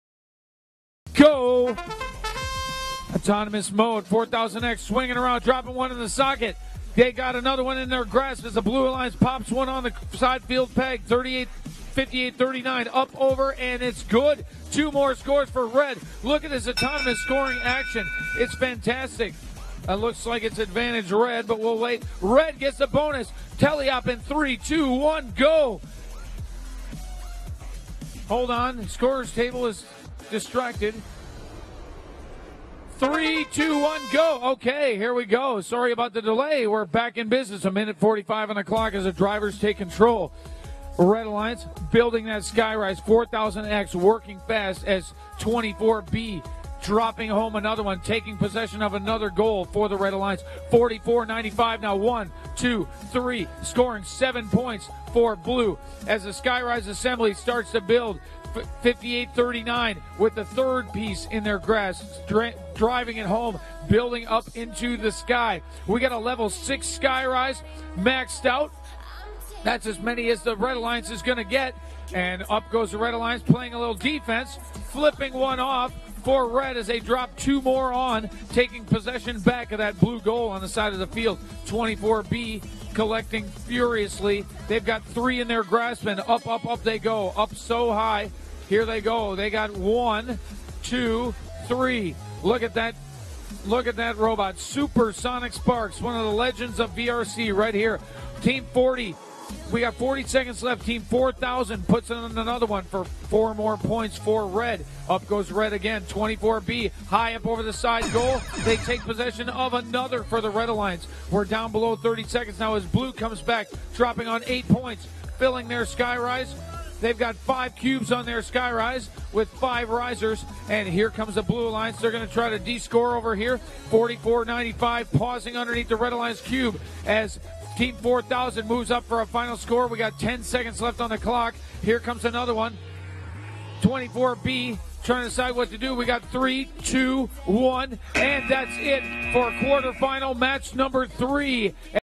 2015 VRC Engr QF3-1 - 24B 4000X 2587 vs 5588R 4495A 5839A - 76 to 55 - VEX Worlds 2015
VEX Worlds 2015 - VRC High School - Engineering Division - Quarter Finals Match 3-1

Red - Score 76:
24B - Super Sonic Sparks B (SPARKS 4-H Club, Westminster, Maryland, United States)

4000X - Omni Eagles (Heritage Academy, Quinlan, Texas, United States)
||  Engineering Rank:3 . Wins:8 . Loses:1 . Ties:1  . WP:17 . SP:395
||  Worlds - Driver Skills: 27 (Rank T-95)

2587 - DiscoBots - Alpha (DiscoBots, Houston, Texas, United States)

Blue - Score 55:
5588R - Calvert Hall Cardinals (Calvert Hall College High School, Towson, Maryland, United States)

4495A - Wolverines A (The Walker School, Marietta, Georgia, United States)

5839A - Tech 1 (Eastern Technical High School, Baltimore, Maryland, United States)